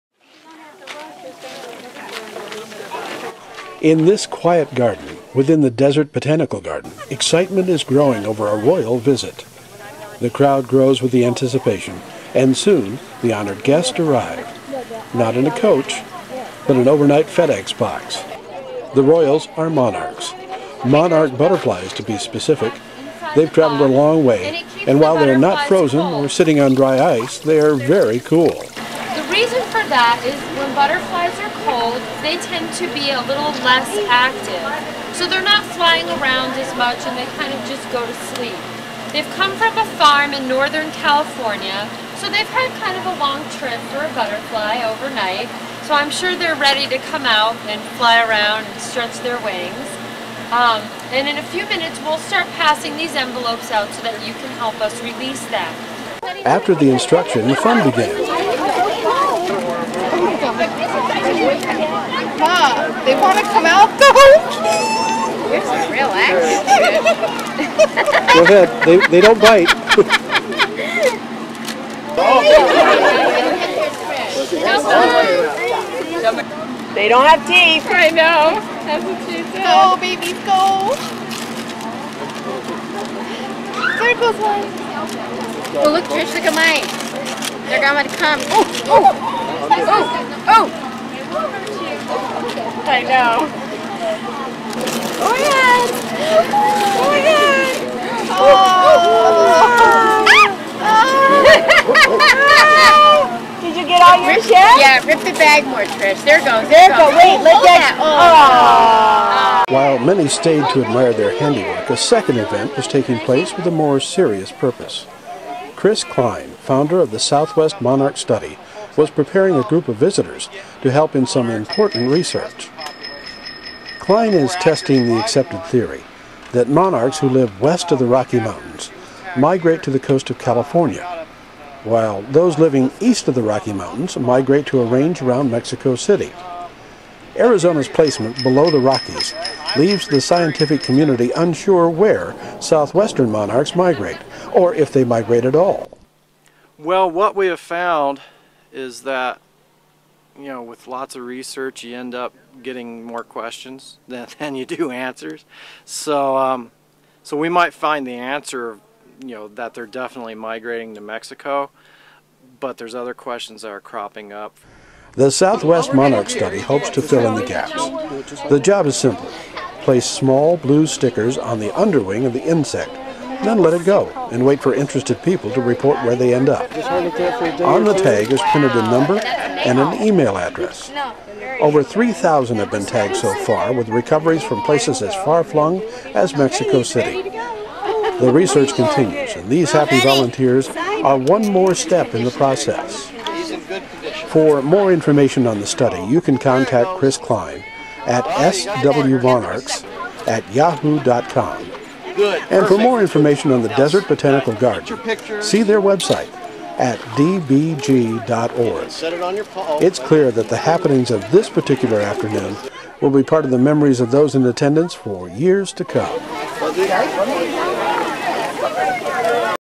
DBG Release
Release and tagging of Monarch butterflies at the Desert Botanical Garden.